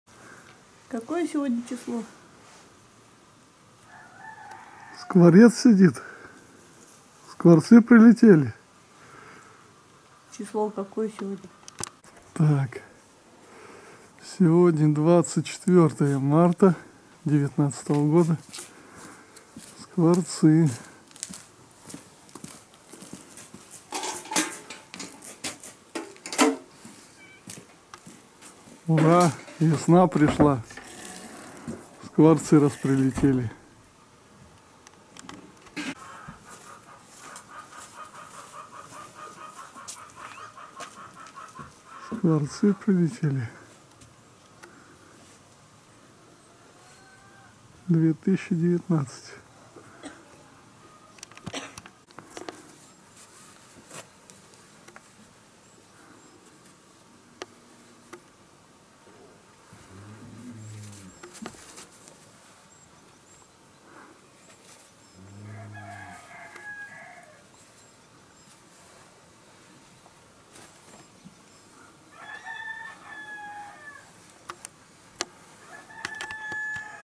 Ура!! Скворцы прилетели, впервые 24 марта 2019 увидел скворца.
Ура!! Скворцы прилетели, впервые 24 марта 2019 увидел скворца. Весна пришла!!!